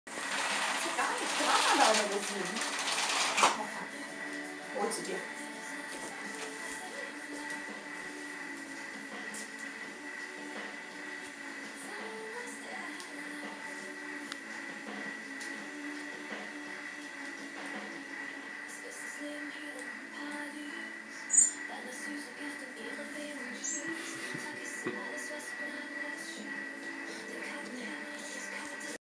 Amsel Fritz beim Rosinenpicken
Fritzchen - unsere Amsel beim morgendlichen Rosinenpicken. Die Amsel folgt uns mittlerweile bis in die Küche.